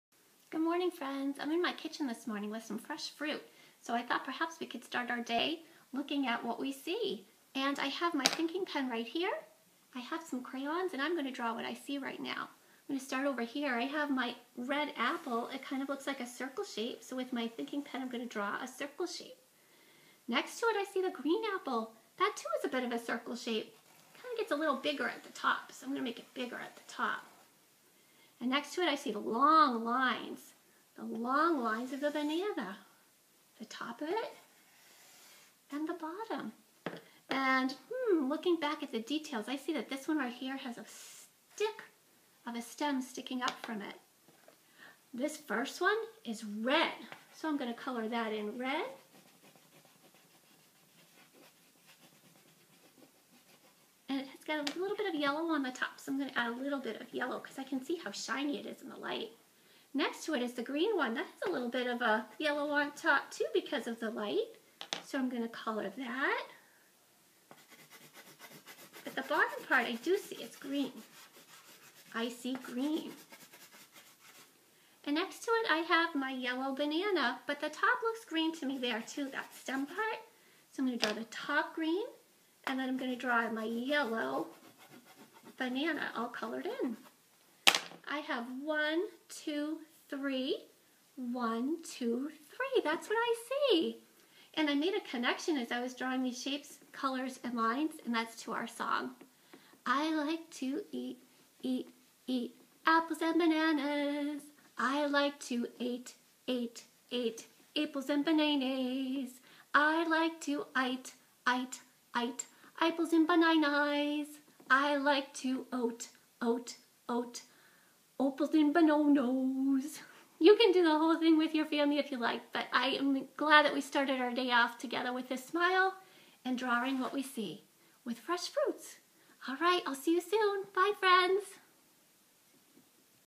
Still life of Apples and Bananas
Using our Thinking Pen, we sketch the fruits.